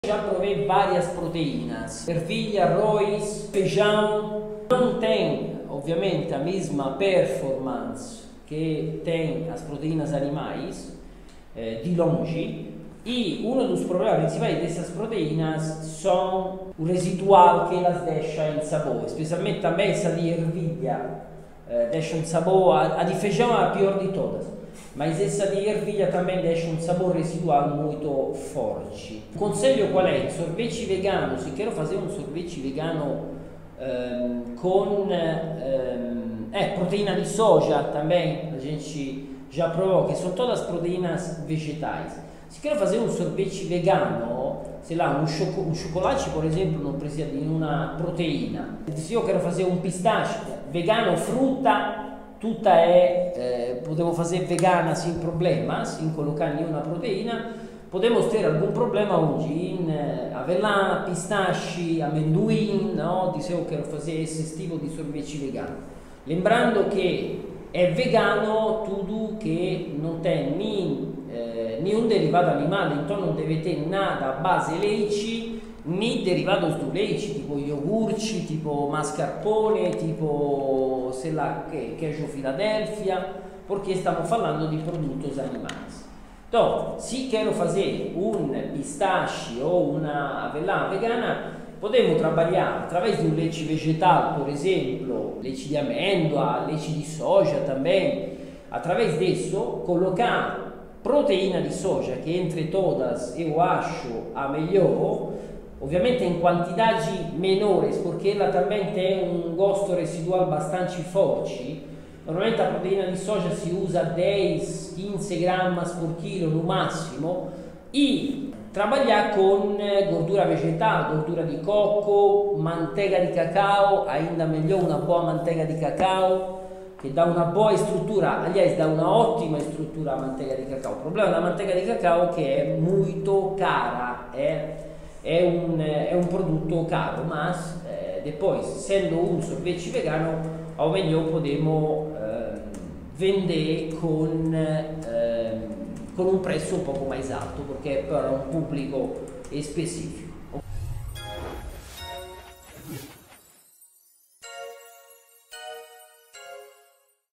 Como substituir proteína animal para fazer sorvete vegano
Se você vai fazer um produto vegano não pode usar nada que seja de origem animal. Mas e se você for fazer um sorvete que precise de proteína animal, o que você pode usar pra substituir?